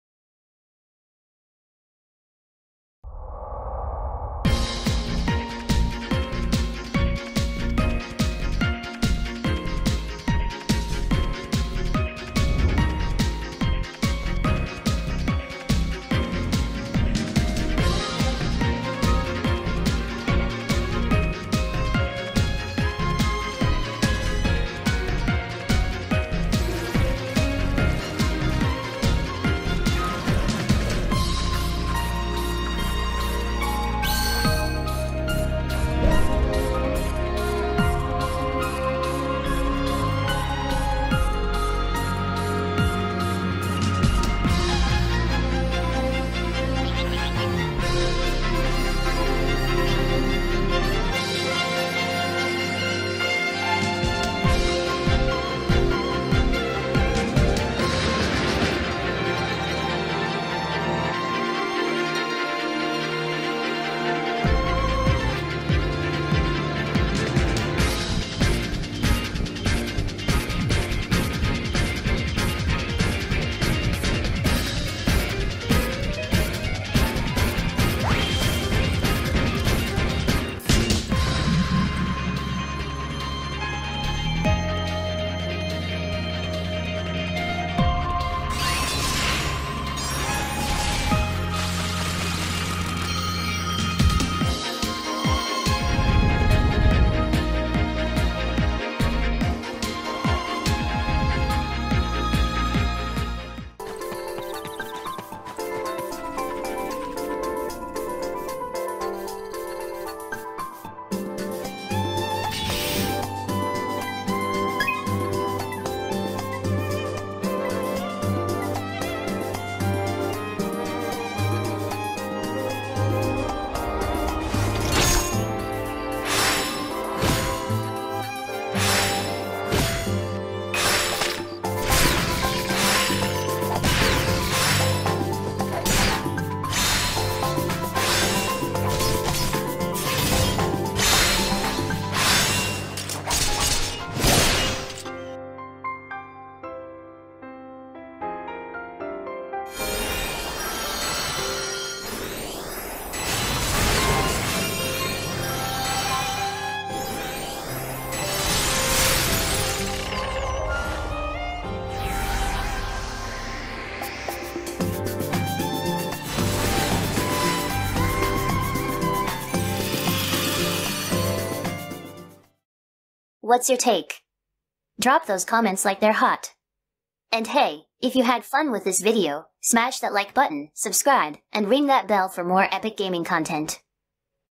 The Legend of Legacy HD Remastered? A Legacy Revisited, Not Reinvented!
Nyanco's Comments :
This game dazzles with its visual upgrade but struggles under the weight of unchanged gameplay and a thin narrative, leaving a mixed legacy of beauty and unfulfilled potential.

Game Overview :
RPG fans rejoice as the classic fantasy of Legend of Legacy is remastered in HD and debuting on Steam!

Full Disclosure :
We were provided a game copy from the developer, publisher or their PR in order to make this content.

Side Note :
Check out what Nyanco Channel has in store for you! (ฅ'ω'ฅ)